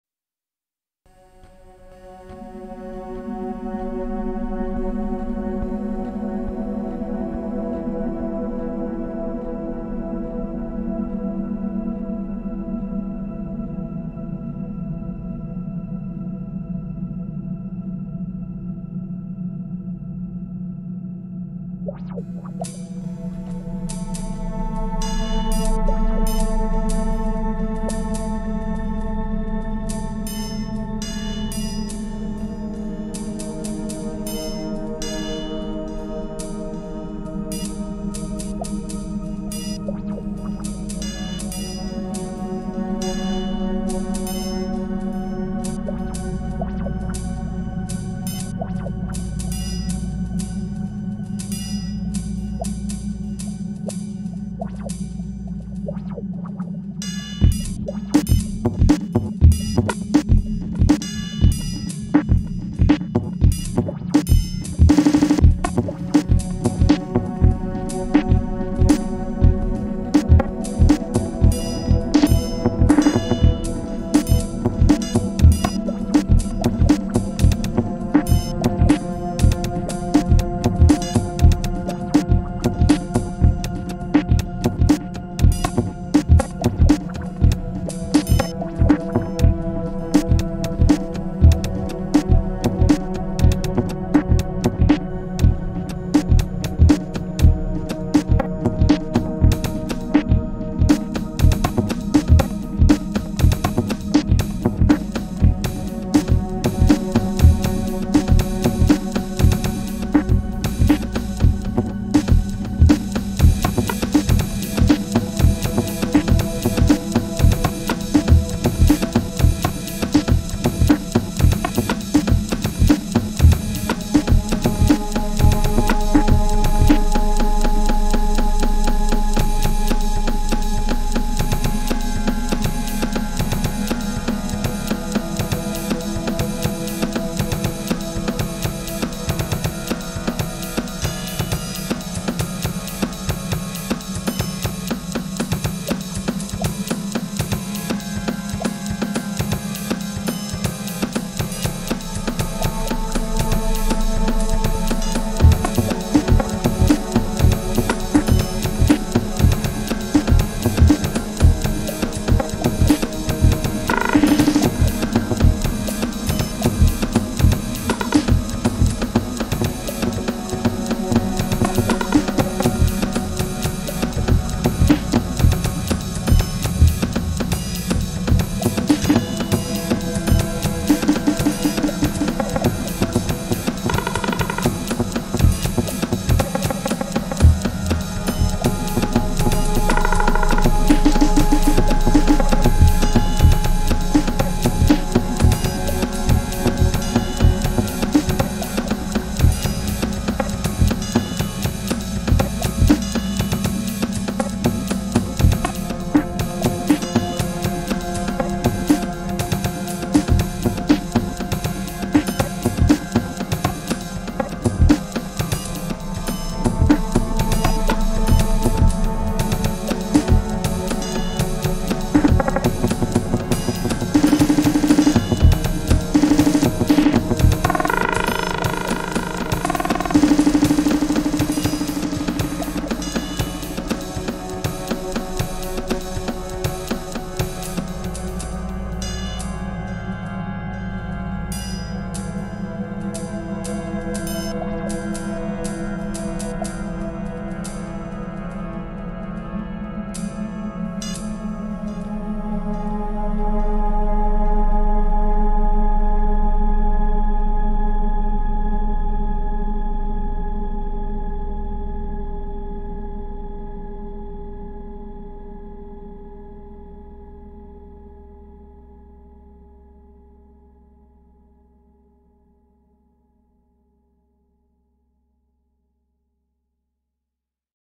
Pads N Beats Model:Samples
Having missed a few weekends was glad to be back for this weeks challenge. Pads and Beats, fun with the Elektron Model:Samples, big pads and crazy drums. Just the way I like it.

In the words of the Sirius Cybernetics Corporation

Kit List : -

Korg NTS-1
Behringer Crave
Behringer TD-3
Novation Circuit
Korg Monotron Delay
Elektron Model:Samples
Korg ES-1 Sampler
Pocket Operator KO 33
Nord Micro Modular
Yamaha SU200
Roland JV-880
novation XioSynth
Yamaha CS 2X

Behringer Flow 8
Phonic Helixboard 12 Firewire Mixer
Phonic 12-2 mixer
Yamaha MV100
Tascam DR40x